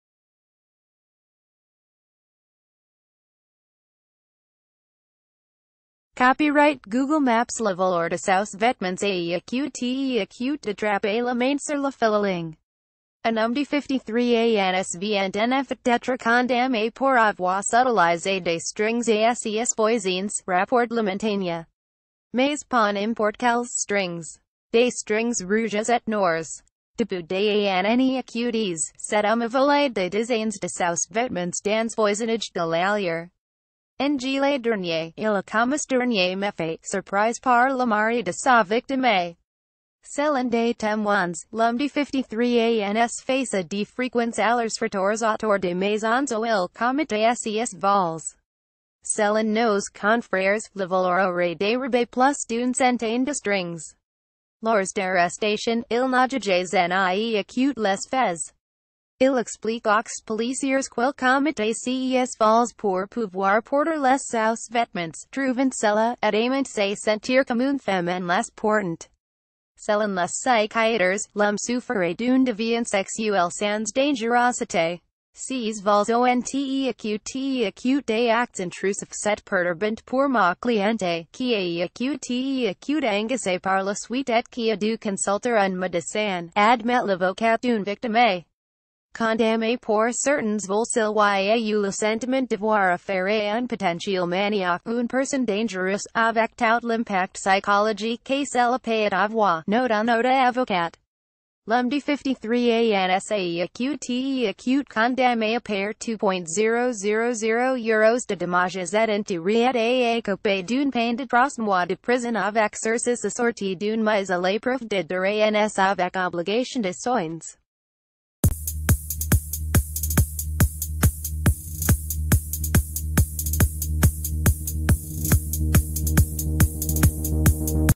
Il volait les strings rouges et noirs de ses voisines
© Google Maps Le voleur de sousvêtements a été attrapé la main sur le fil à linge. Un homme de 53 ans vient en effet dêtre condamné pour avoir subtilisé des strings à ses voisines, rapporte La Montagn...